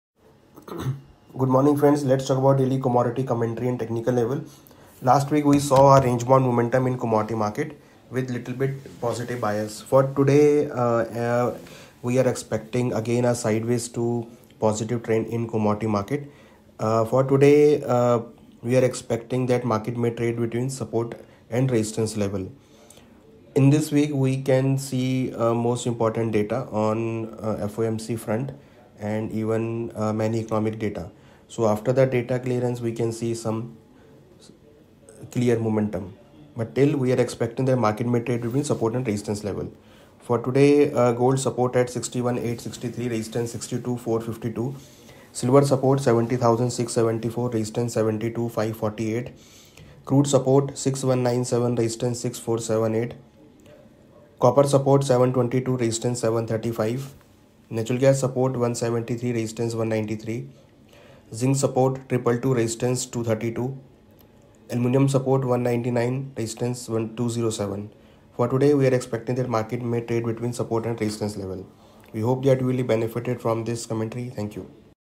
Daily Commodity Market Outlook 29.01.2024 by Mr. Anuj Gupta, HDFC Securities
Daily Commodity Market Outlook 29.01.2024  Mr. Anuj Gupta,  HDFC Securities

SEBI Registration number is INZ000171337